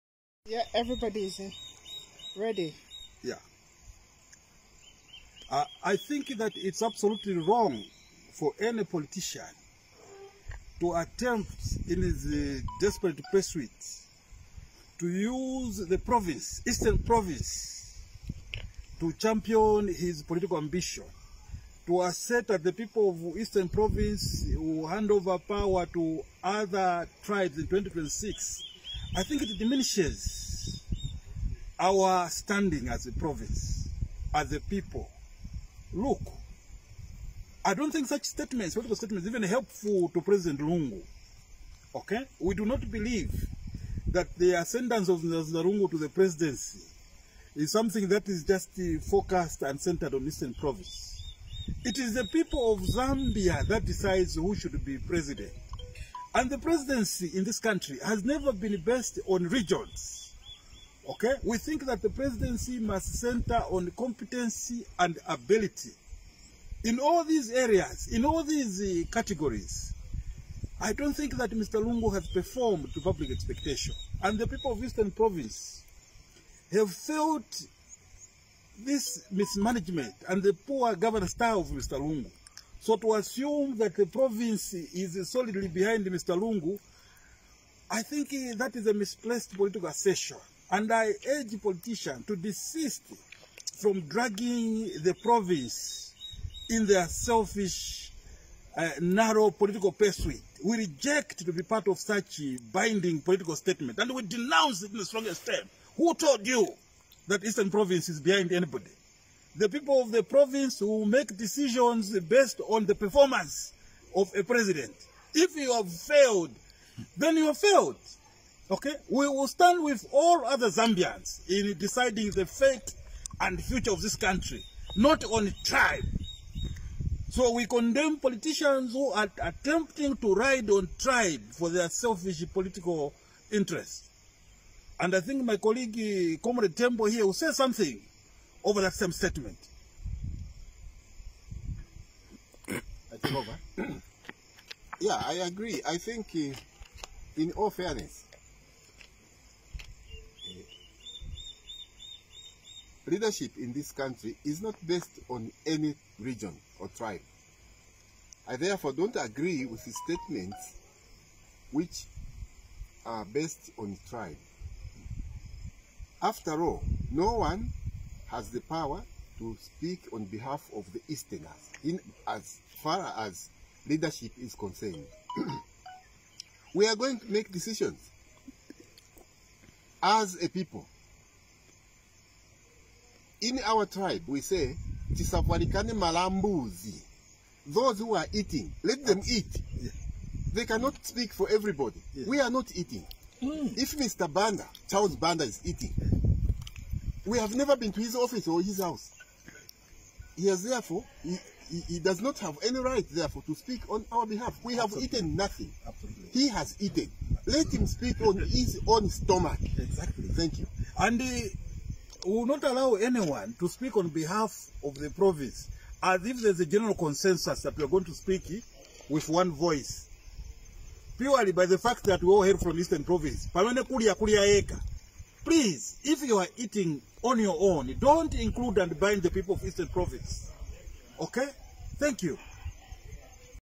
Easterners say in 2021 they are voting based on performance & not that they r behind president Lungu
The people of Eastern province have declared that in 2021 their votes will be based on performance. They have charged that it is a misplaced political Assertion that Eastern province is solely behind president Edgar Lungu.